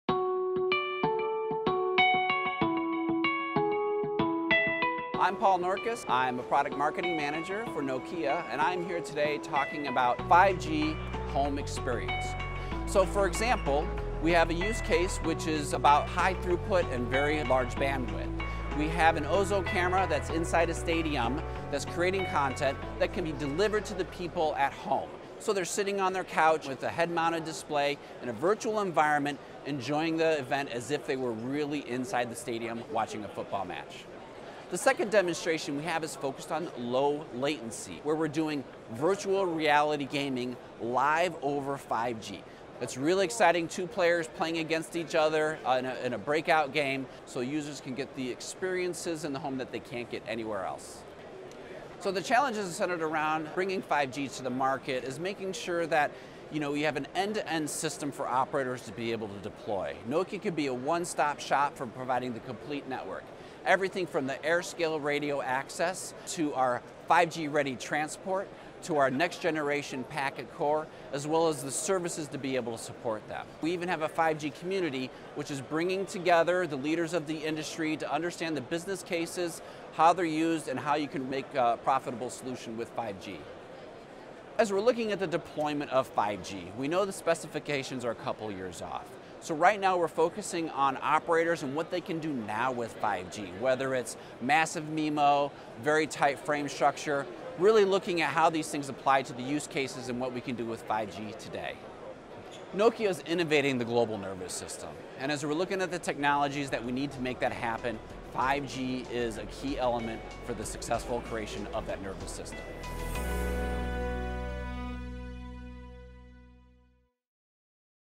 Commercial 5G FIRST & 5G for Home
See the 5G FIRST commercial solution deliver ultra-broadband to the home enabling new experiences like virtual reality.